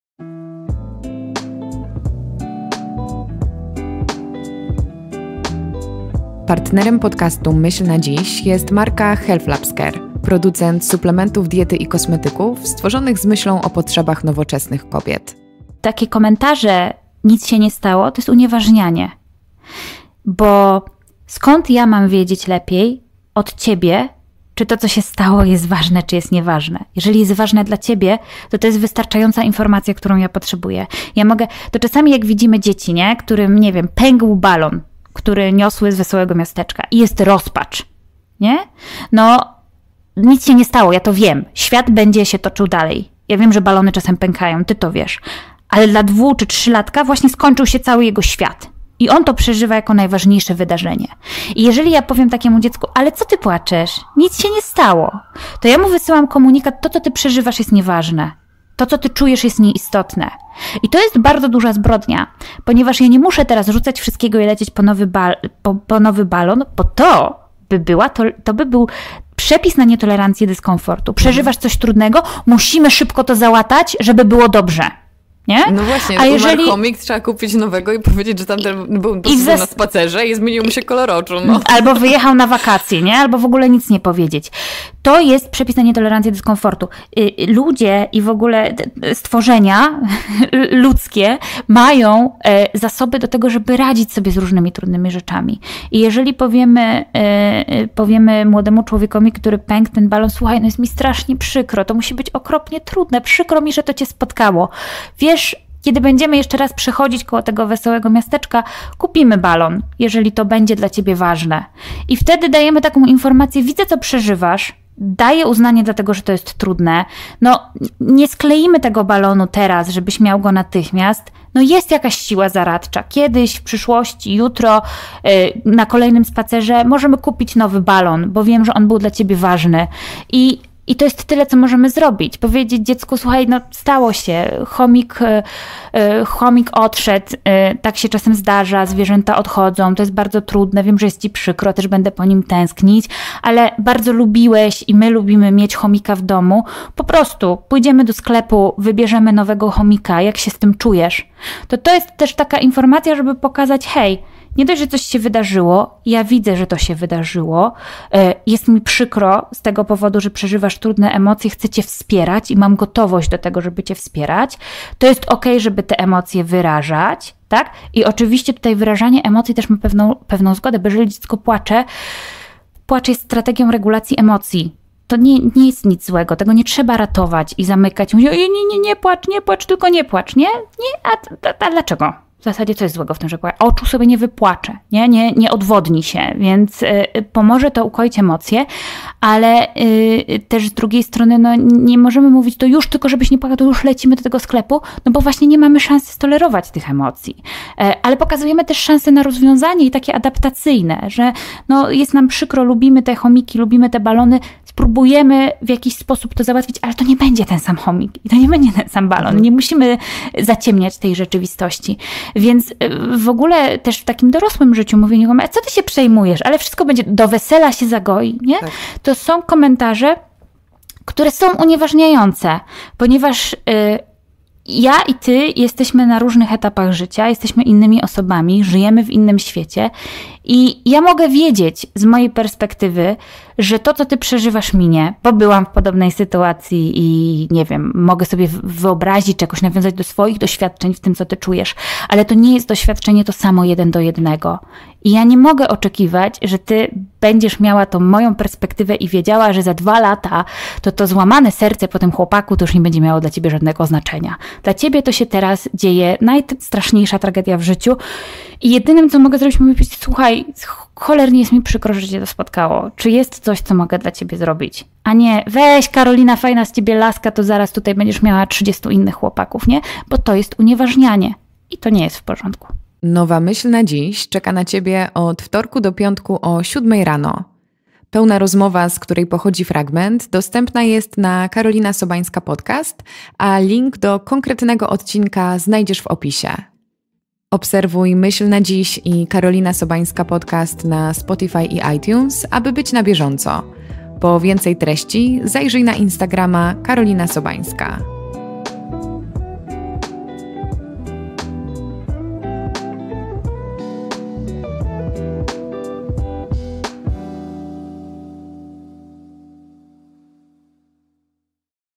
toleruj dyskomfort / myśl na dziś
Dowiedz się dlaczego powinniśmy nauczyć się tolerować negatywne emocje i o tym, jak pocieszać innych, żeby nie unieważniać tego, co czują.

Montaż odcinków:
Eugeniusz Karlov